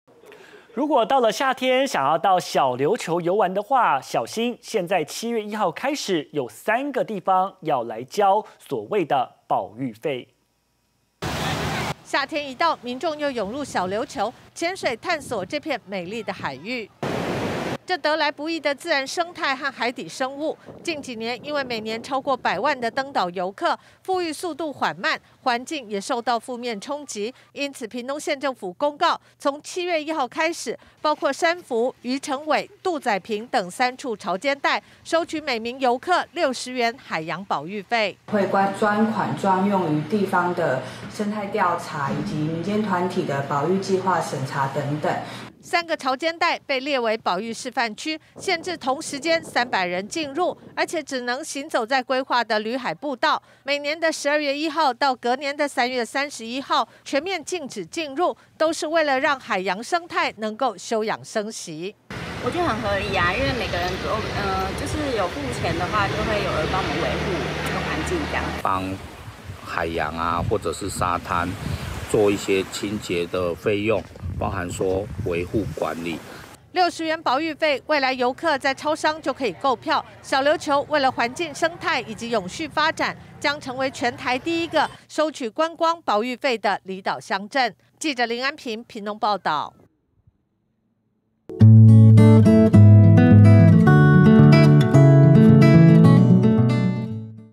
保護海洋！ 小琉球3潮間帶7／1起每人收「60元保育費」
屏東離島小琉球是熱門觀光景點，近幾年登島的遊客人數都破百萬，也使得當地的海洋生態受到負面衝擊，屏東縣政府為了生態永續，從7月1日開始，遊客如果要進入三個被列為示範區的潮間帶，都必須要繳交每人60元的海洋保育費，未來遊客在超商就可以購票。小琉球為了環境生態以及永續發展，將成為全台第一個收取觀光保育費的離島鄉鎮。